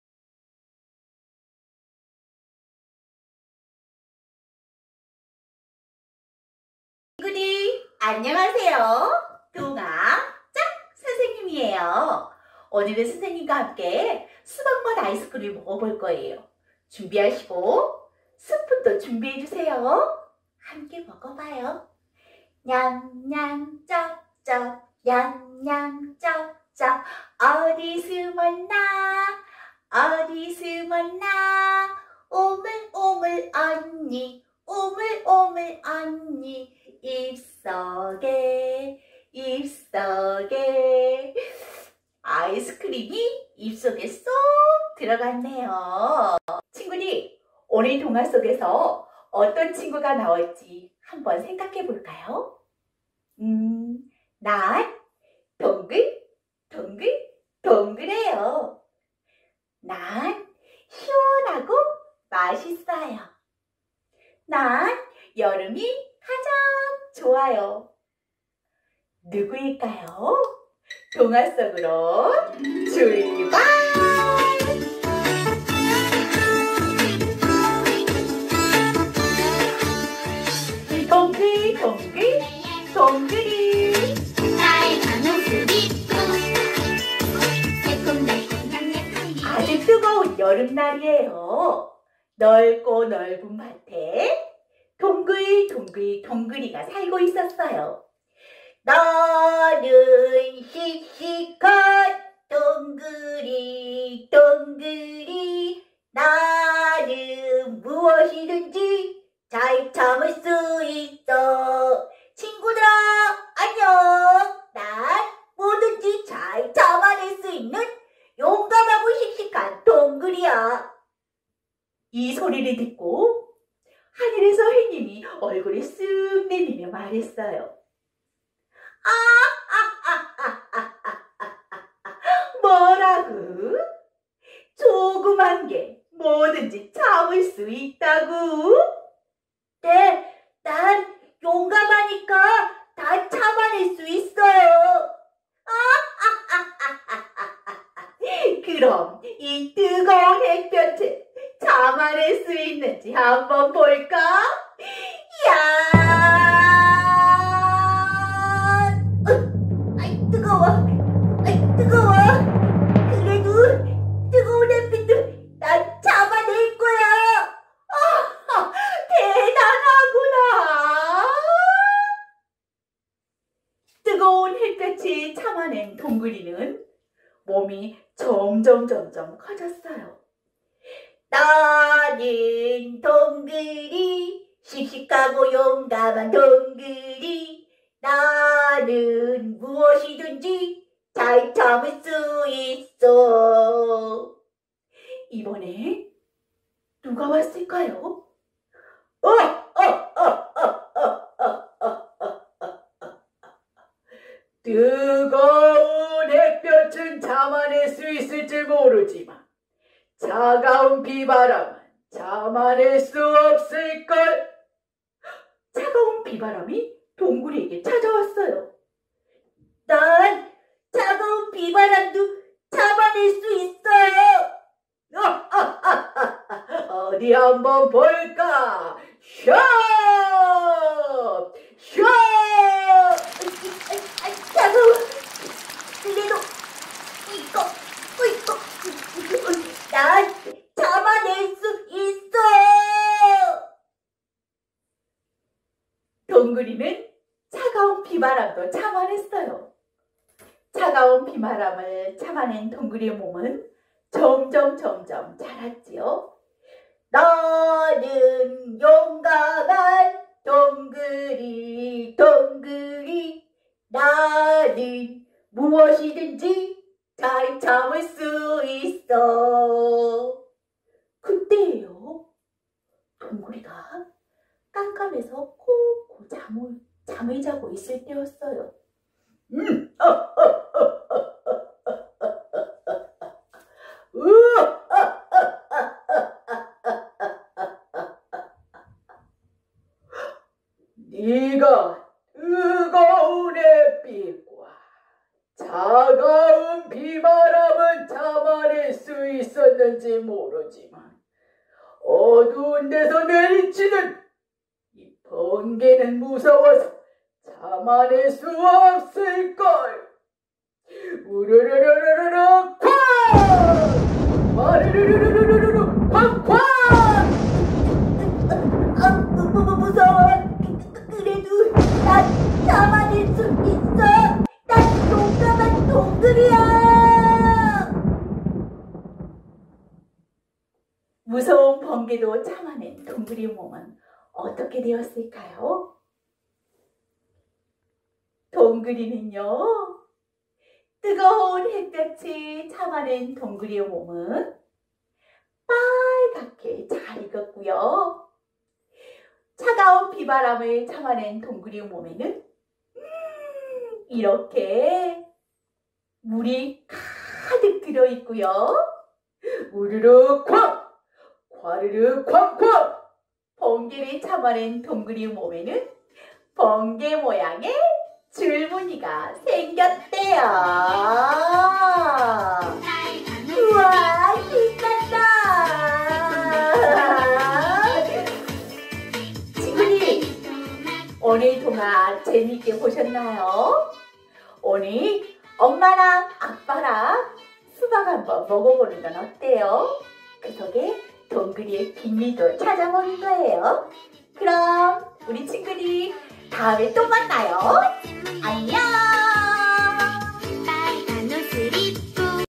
[아무때나 온라인 강좌] 신나는 동화나라(8)-동글동글 동글이
★ 동화구연 선생님과 함께하는 낚시줄동화 「동글동글 동글이」에 눈은 반짝, 귀는 쫑긋~!